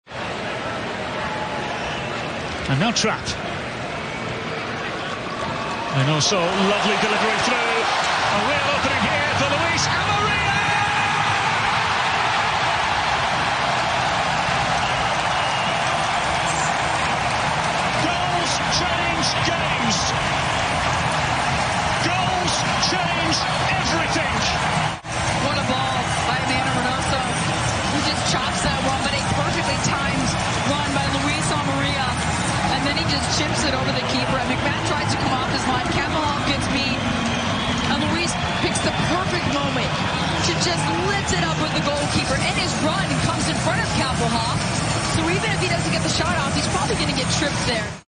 GOAL: Luis Amarilla, Minnesota United FC - 61st minute
Luis secures a third goal for the Loons with a cheeky chip in the 61st minute.